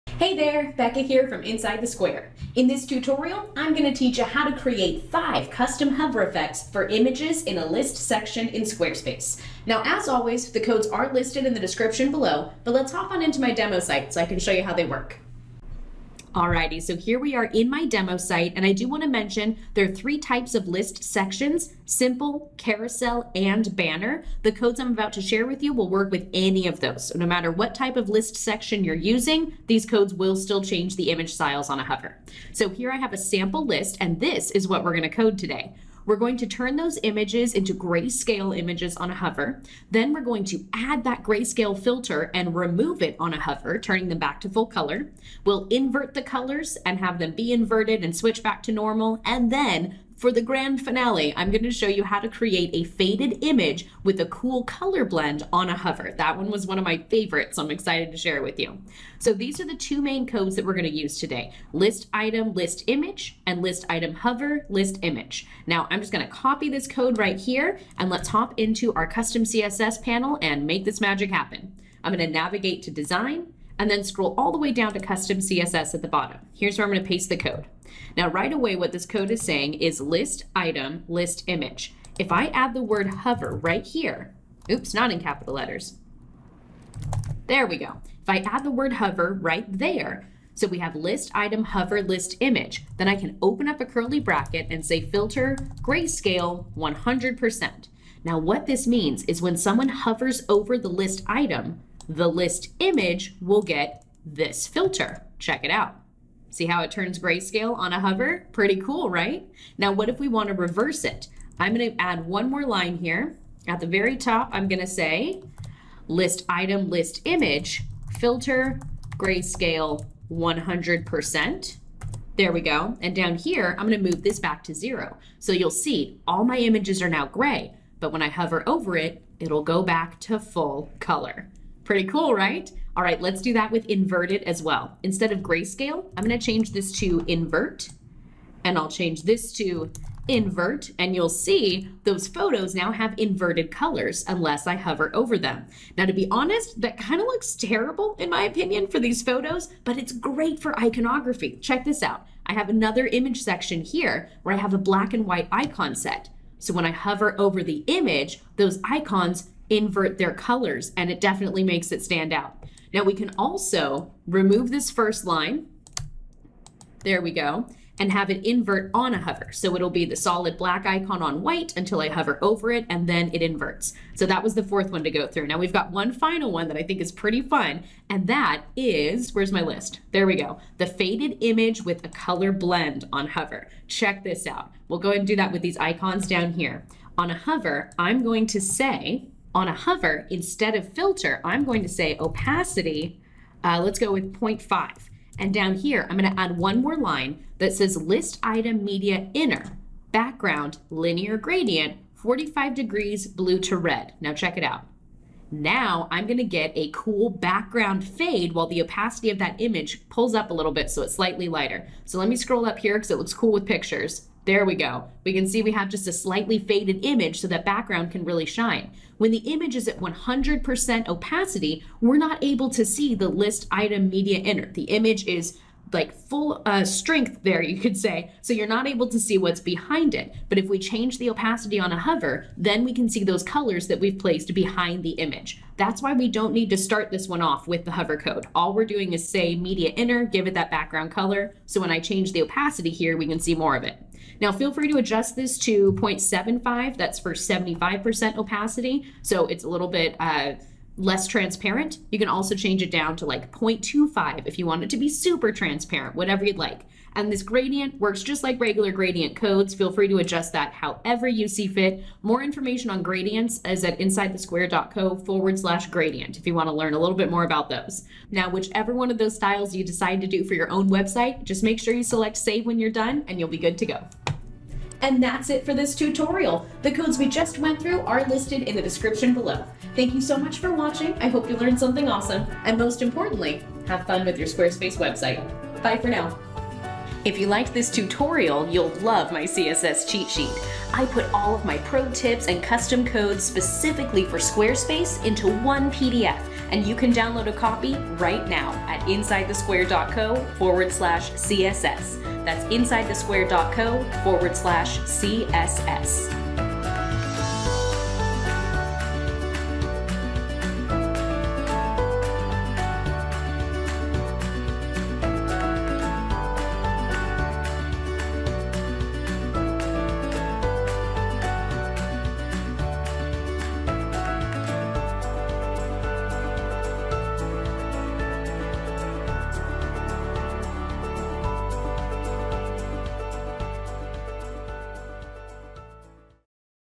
5 List Image Hover Effects in Squarespace // List Section for Squarespace 7.1 Tutorial
In July 2022, Squarespace made a big change to the way sites are edited, and this tutorial is a little outdated! For the latest info, check out
In this tutorial, you’ll learn how to create 5 unique hover effects for a list item image in Squarespace 7.1

A few pro tips before you dig in:

⚠️ This is specifically for version 7.1 - older versions of Squarespace don’t have list sections, but I do have another tutorial on image hover effects you can check out here. It’s an old one but a good one!

✨ There is more than one way to do this! This is just one approach of many because code is super customizable.

🔗 Here is a link to my list of image filters that work with Squarespace

📄 You can install these on a single page using page header code injection or a code block in a section above or below the list section. Here is a link to a tutorial on how to set that up

🙋 If you need any help along the way, head on over to see my current support options.

Here are the codes from this tutorial. Have fun with your Squarespace website!

//-- Greyscale on hover
 .list-item:hover .list-image {filter: grayscale(1);}

//-- Grayscale to full color on hover
 .list-item .list-image {filter: grayscale(1);}
 .list-item:hover .list-image {filter: grayscale(0);}

//-- Inverted colors to normal on hover
 .list-item .list-image {filter: invert(100%);}
 .list-item:hover .list-image {filter: invert(0%);}

//-- Inverted colors on hover
 .list-item:hover .list-image {filter: invert(100%);}

//-- Color blend on hover
 .list-item:hover .list-image {filter: grayscale(1); opacity:.5}
.list-item-media-inner {background: linear-gradient(45deg, blue, red)!important}